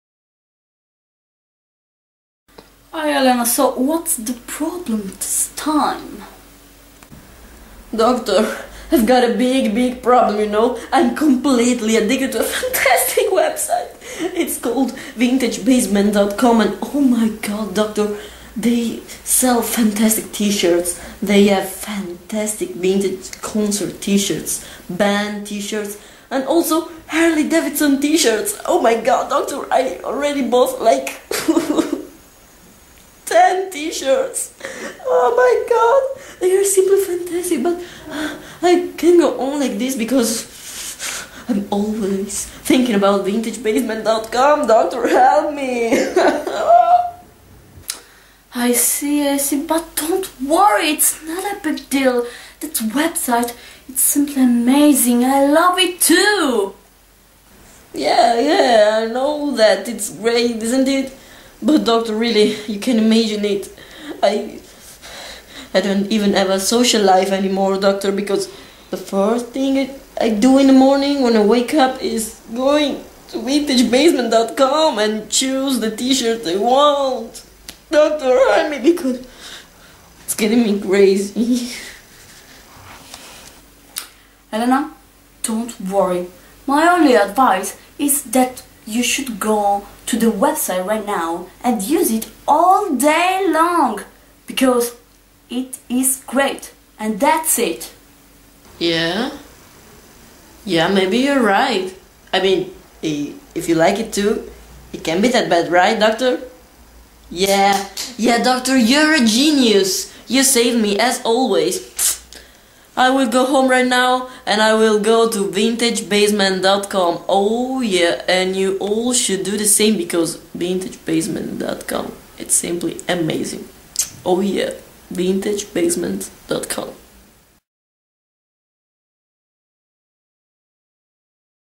Crazy for Vintage Basement
Hot Italian chick is crazy for Vintage Basement's t-shirts. She talks with her psychiatrist about her obsession with our great t-shirts. Go to Vintage Basement and see why she's obsessed!